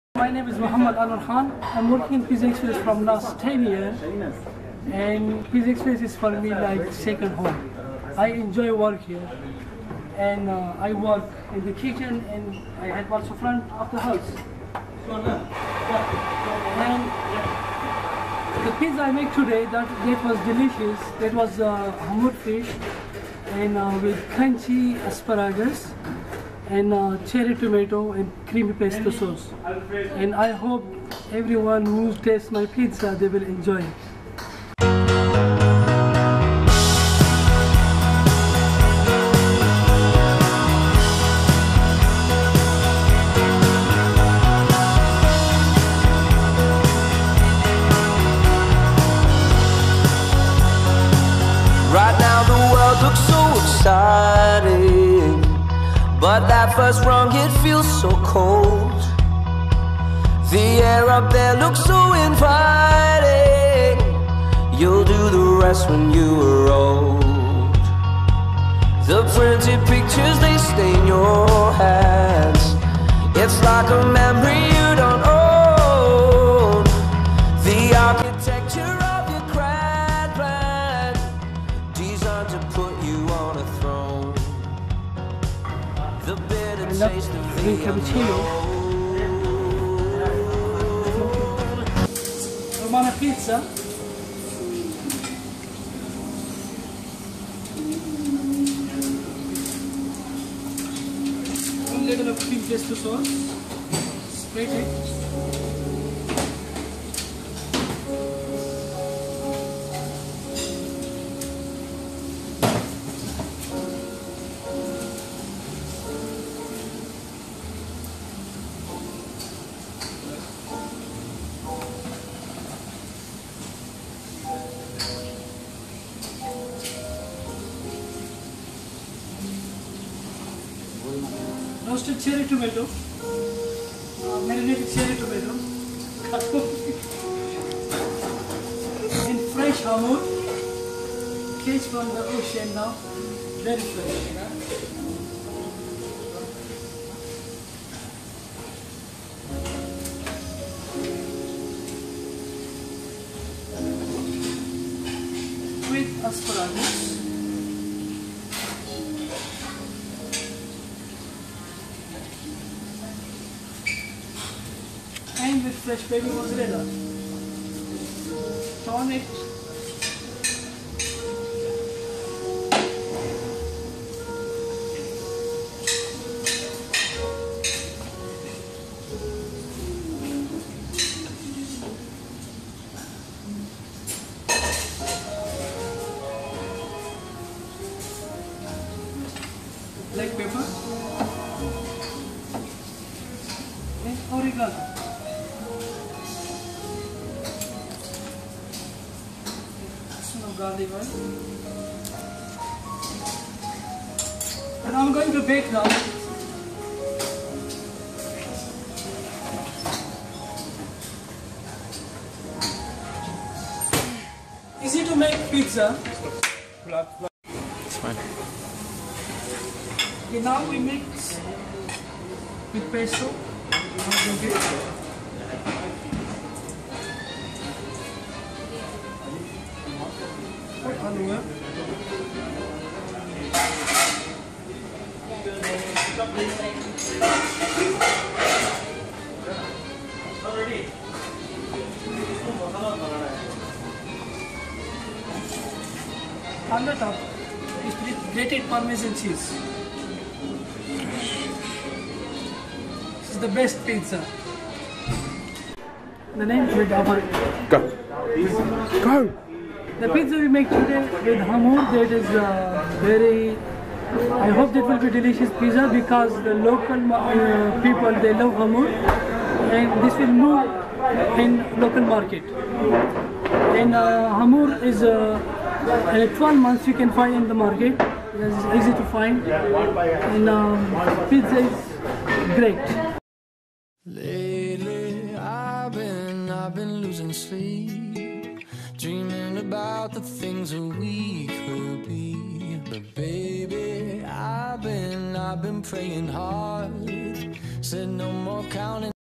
Mohammad Anwar Khan Shah|PizzaExpress UAE| Pizzaexpress UK | Pizzaiollo of the year|Pizzaexpress Jaz
Mohammad Anwar Khan
from UAE
awarded the best Chef over 400 restaurant in Pizzaexpress globally